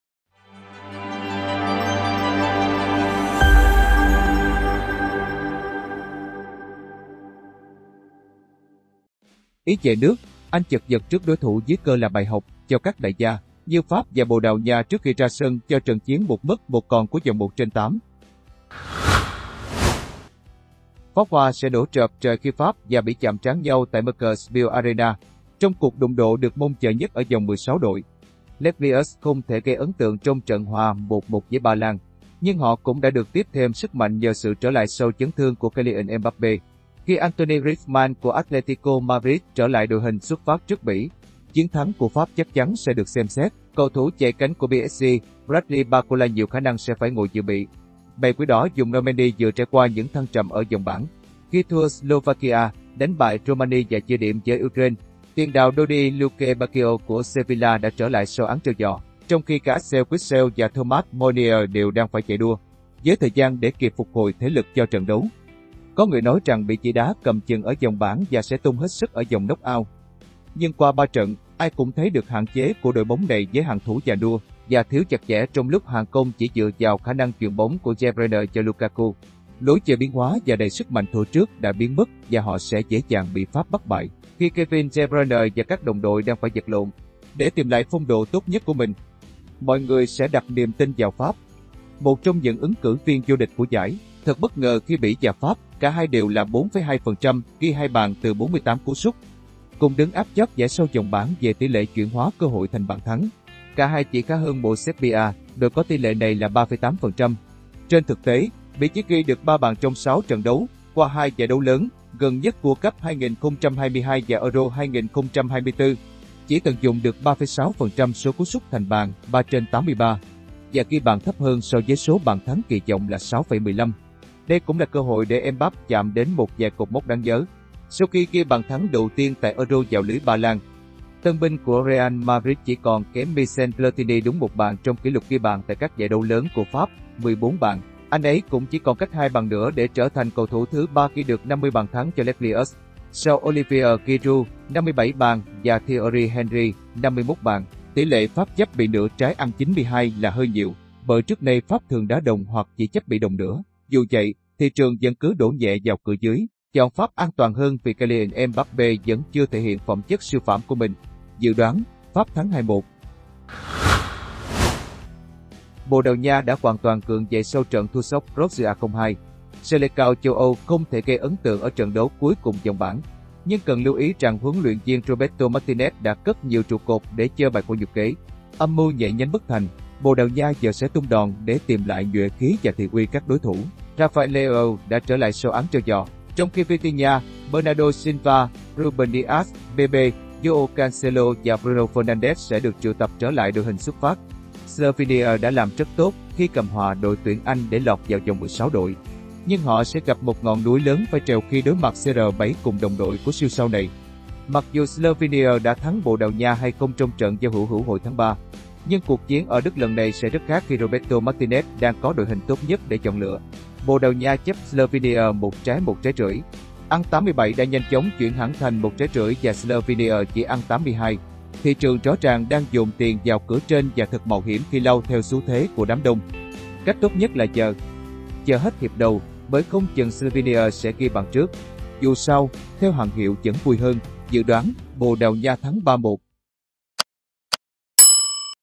Euro 2024: Soi tỉ số Pháp – Bỉ và Bồ Đào Nha – Slovenia | Ngoài đường piste
Ý về nước, Anh chật vật trước đối thủ dưới cơ là bài học cho các "đại gia" như Pháp và Bồ Đào Nha trước khi ra sân cho trận chiến "một mất một còn" của vòng 1/8.

Thực hiện: Vệ Loan

© Kênh Youtube chính thức của Ngoài Đường Piste. Toàn bộ clip thuộc quyền sở hữu của Ngoài Đường Piste.